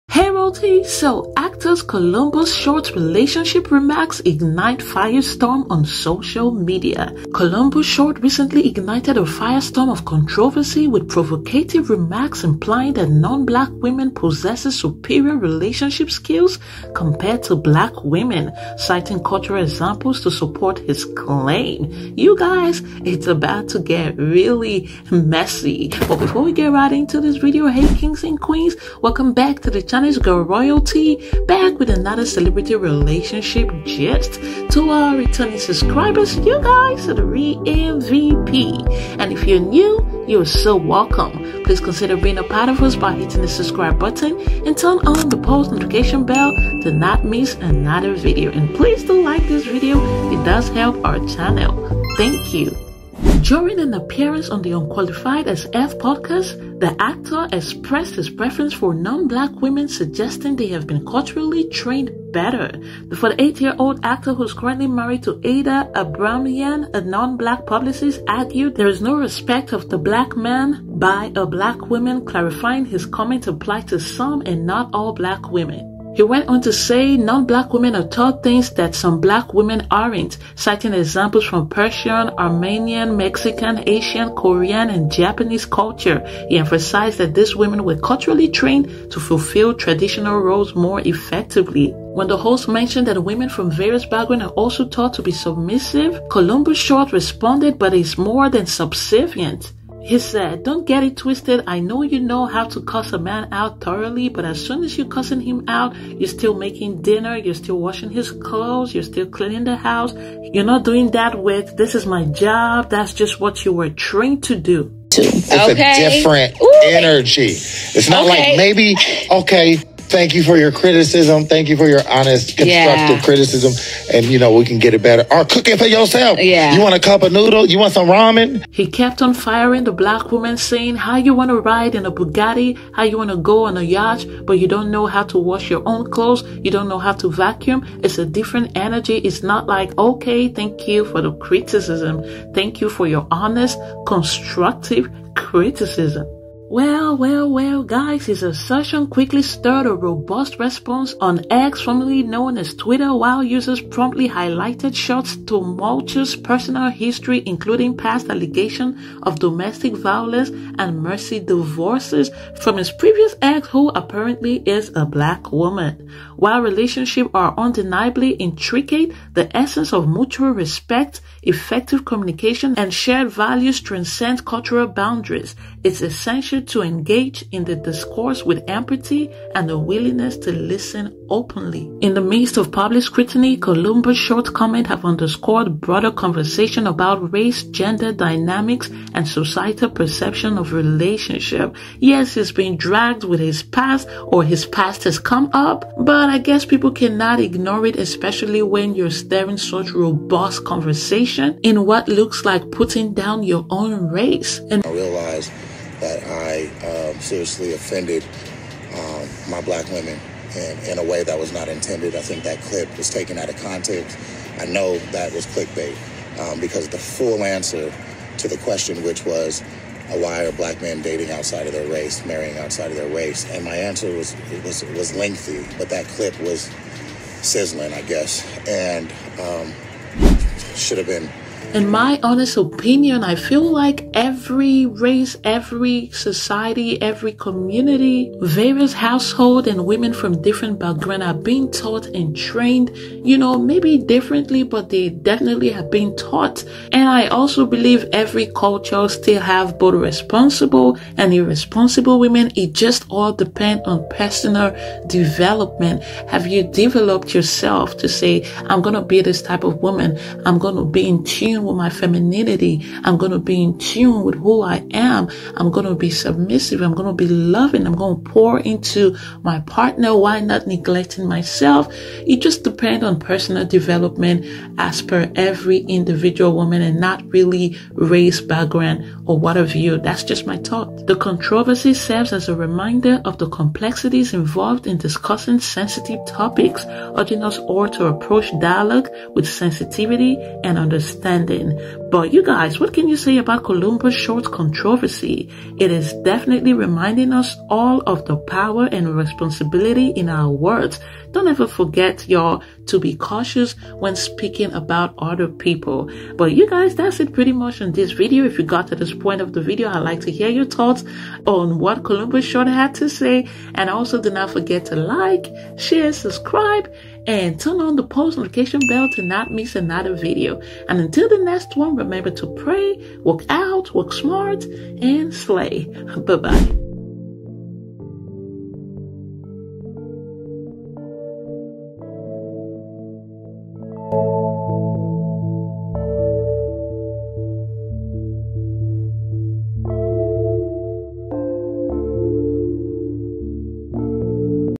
Columbus Shorts Apologizes After Being Dragged For His Relationship Remarks towards Black Women
columbusshort apologises after making generalizayion about black women, and he was being dragged by online users, who brought up his past misdermeanor. Did his new statement make it any better or was he out of line again?

If you are currently experiencing DA and need immediate help call the 24-hour DV Hotline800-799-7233